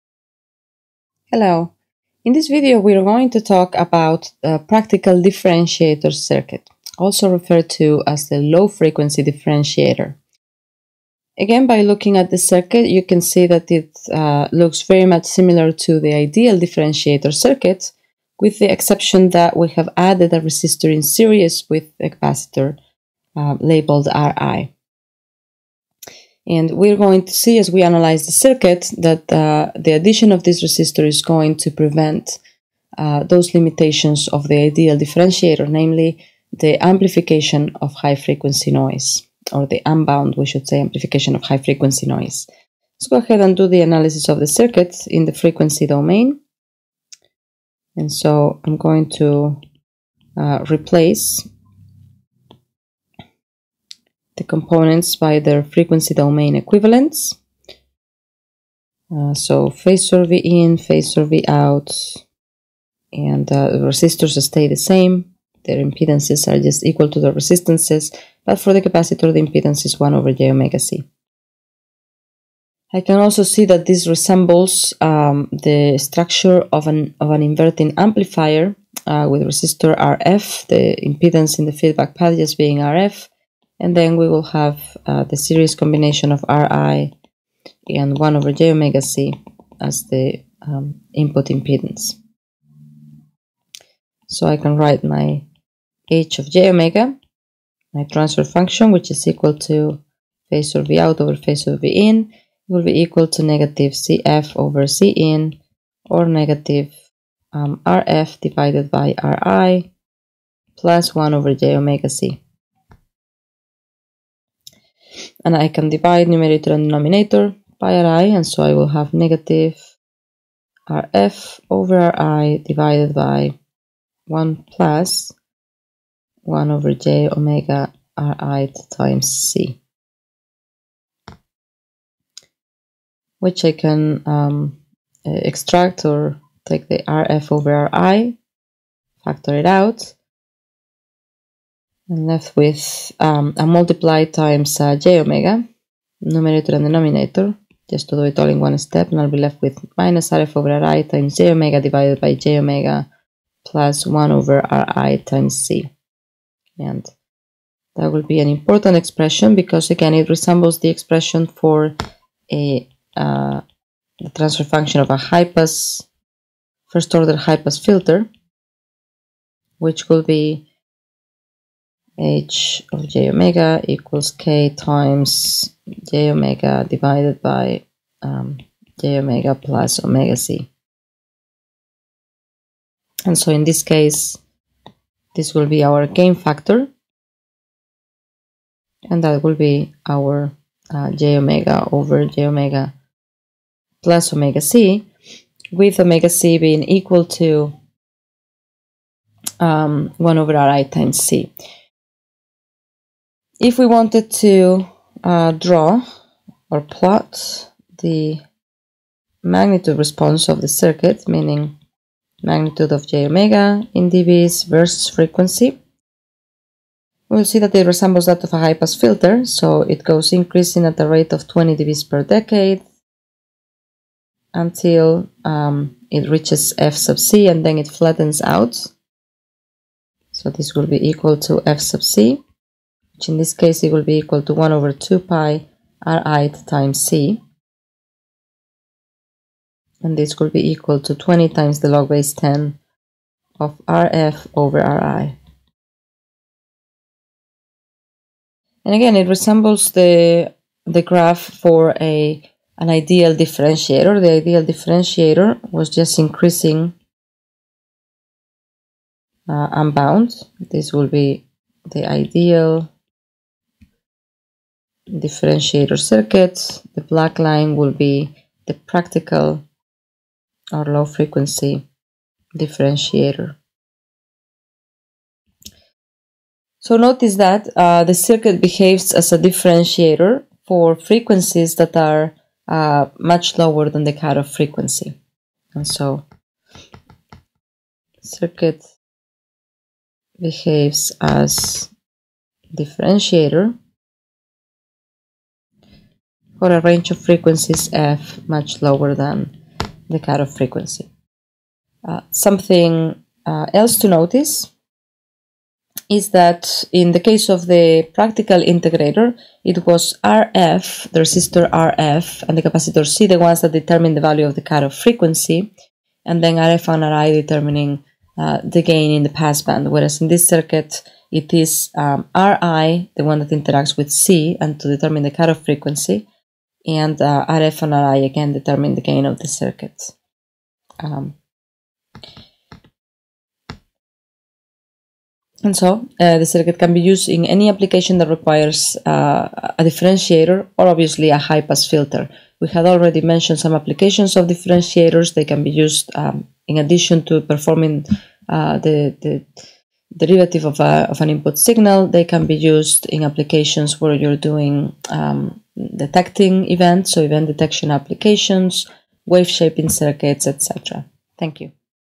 Practical differentiator
Analysis of the practical differentiator (active first-order highpass filter)